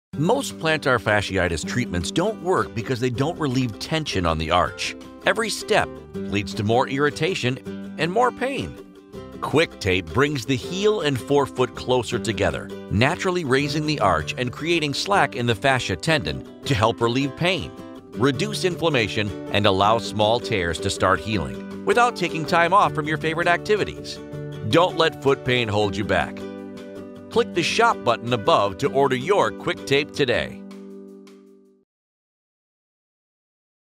Why Quick Tape Works Better (and Faster) to Heal Foot Pain
What makes Quick Tape different from standard taping, sleeves, straps, and other products for plantar fasciitis? It's all in the design!  Quick Tape is a one-piece strap made from a medical grade self-adhesive, non-stretch athletic tape. It actually lifts the arch, bringing the forefoot and heel closer together and giving slack to the plantar fascia, so it can effectively rest and tiny tears can heal. Quick Tape customers experience quick, often instant, pain relief!